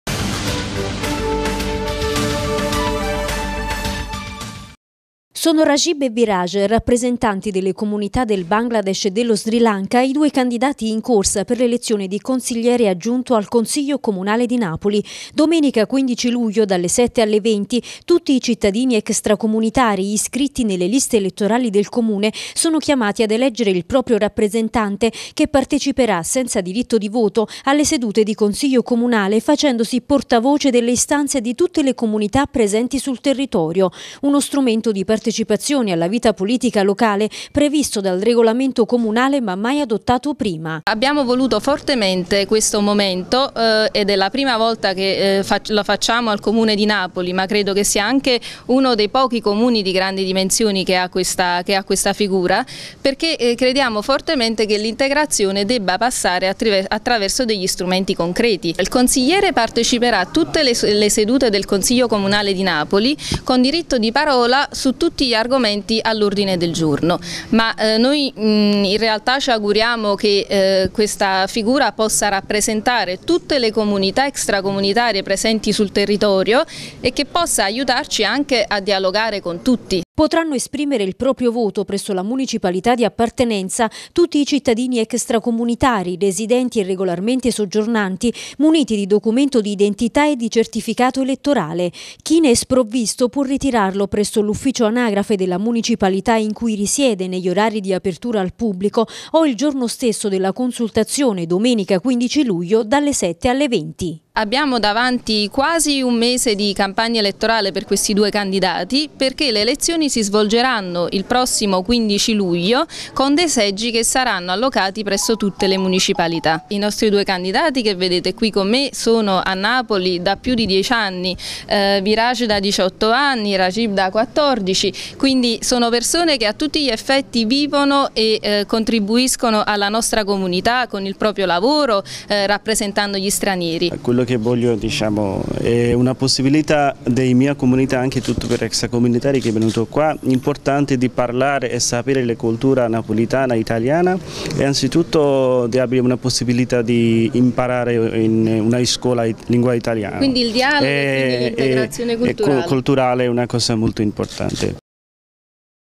Per la prima volta un Consigliere extracomunitario, si vota il 15 luglio
Presentati i due candidati extracomunitari alla carica di Consigliere Aggiunto in Consiglio Comunale. Si voterà presso le Municipalità, domenica 15 luglio, dalle 7 alle 20.